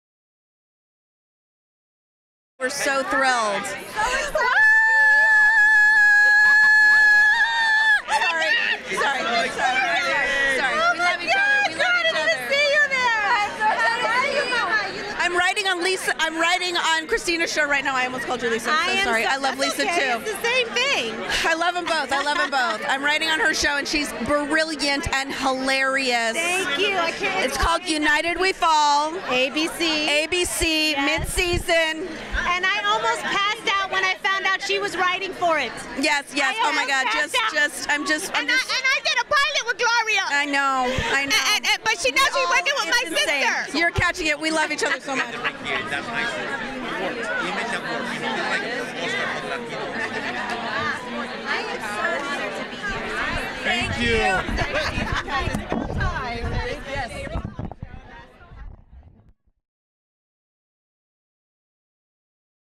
BTS | A Red Carpet Moment with Gloria Calderon Kellett
Hispanic Lifestyle caught a moment with executive producer Gloria Calderon Kellett and sisters Lisa and Christina Vidal on the red carpet at the 2019 Imagen Foundation Awards.